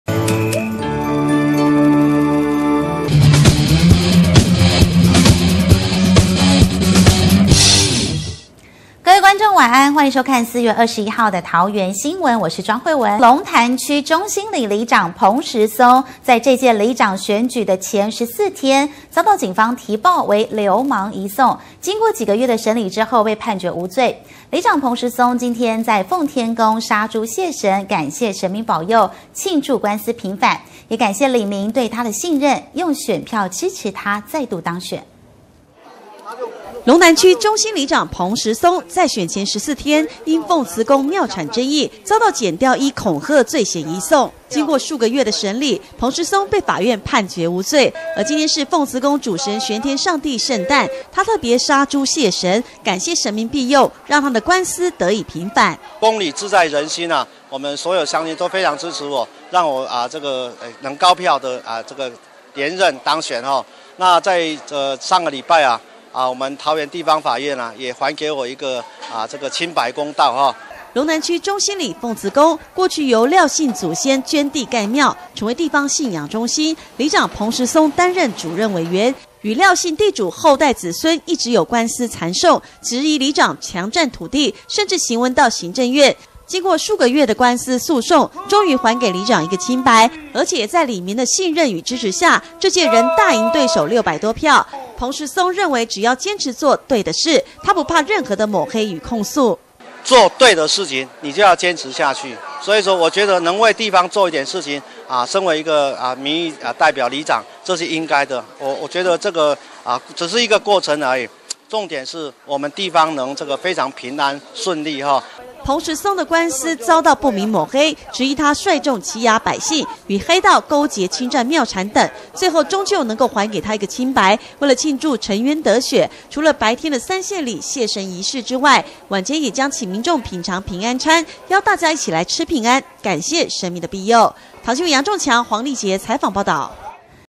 南桃園地方新聞 2015 04 21彭石松里長感恩
轉載:南桃園地方新聞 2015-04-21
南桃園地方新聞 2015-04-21
龍潭里長彭石松官司平反 感謝神明庇佑
龍潭區中興里長彭石松在這屆里長選舉前14天，遭警方依恐嚇罪嫌移送，經過幾個月的審理後，被判決無罪；里長彭石松今天在奉天宮殺豬謝神，感謝神明保佑、慶祝官司平反，也感謝里民對他的信任，用選票支持他再度當選。
龍潭區中興里長彭石松在選前14天，因奉慈宮廟產爭議，遭檢調依恐嚇罪嫌移送，經過數個月的審理，彭石松被法院判決無罪，而今天是奉慈宮主神玄天上帝聖誕，他特別殺豬謝神，感謝神明庇佑，讓他的官司得以平反。
龍潭區中興里奉慈宮過去由廖氏祖先捐地蓋廟，成為地方信仰中心，里長彭石松擔任主任委員，與廖氏地主後代子孫一直有官司纏訟，質疑里長強佔土地，甚至行文到行政院，經過數個月的官司訴訟，終於還給里長一個清白，而且也在里民的信任與支持下，這屆仍大贏對手600多票，彭石松認為只要堅持作對的事，他不怕任何的抹黑與控訴。
彭石松的官司遭到不明抹黑，最後終究能夠還他一個清白，為了慶祝澄冤得雪，除了白天的三獻禮謝神儀式外，晚間也將請民眾品嚐平安餐，邀大家一起來吃平安，感謝神明庇佑。桃園新聞 黃麗婕 楊仲強